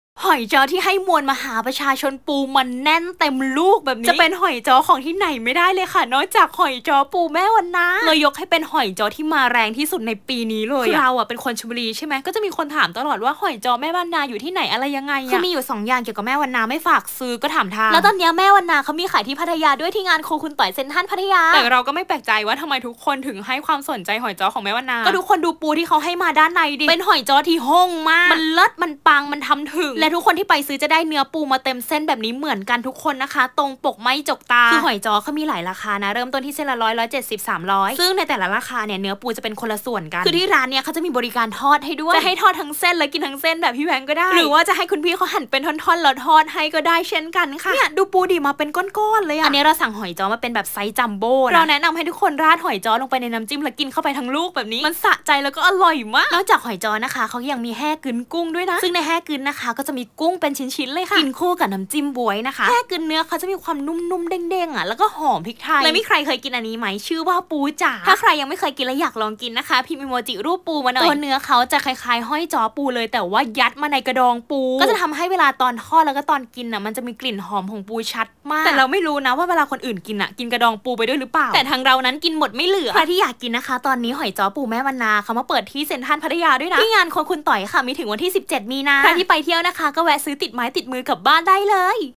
หอยจ๊อปูที่มาแรงแซงทุกโค้งสุดในช่วงนี้ "หอยจ๊อปูแม่วรรณา" 🦀🦀🦞🦞
หอยจ๊อปูที่มาแรงแซงทุกโค้งสุดในช่วงนี้ ต้องร้านนี้
เลย ที่สุดแห่งปู คนถามหลังไมค์มาเยอะมากก เจอ
ได้ที่งาน ครัวคุณต๋อยออนเดอะบีช เซนทรัลพัทยา

"หอยจ๊อปูแม่วรรณา" 🦀🦀🦞🦞